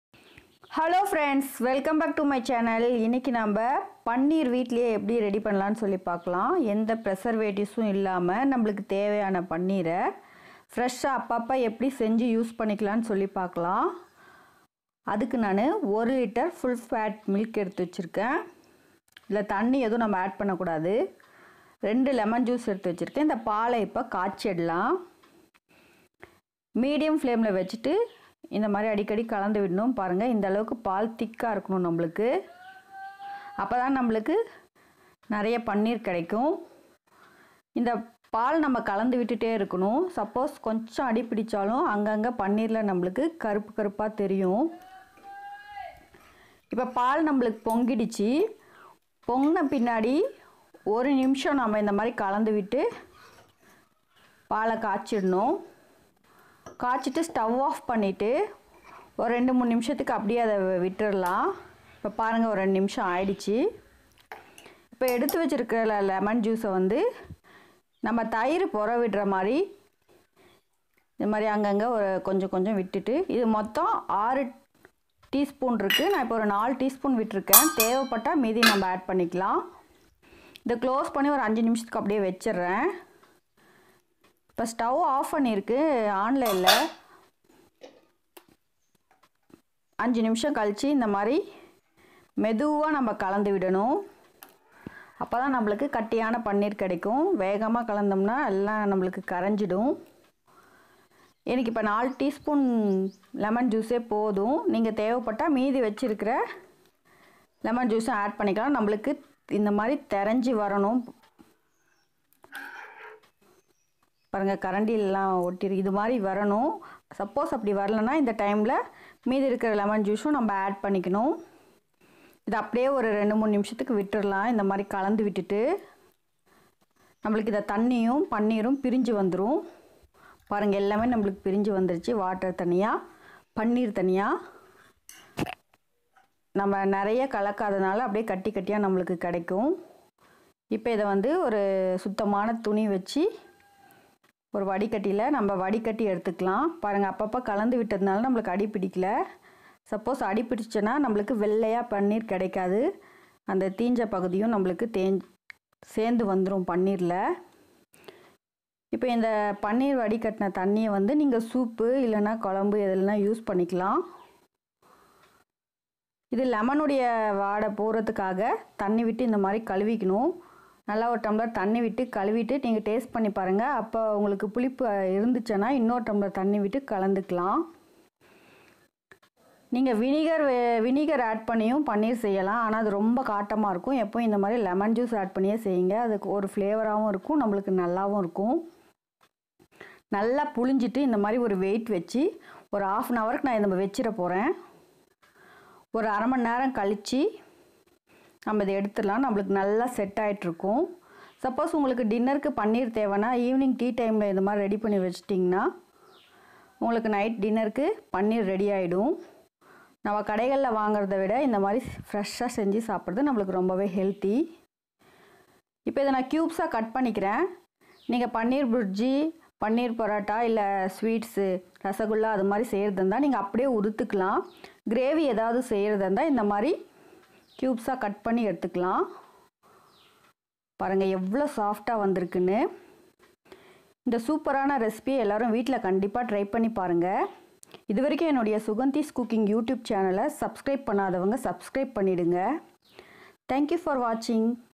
பன்னீர் செய்முறை | Homemade Paneer | Suganthis Cooking.
This channel is about to Cooking and Gardening tips for beginners.

INGREDIENTS :

Full Fat Milk   - 1 liter

Lemon Juice  - 6 Teaspoons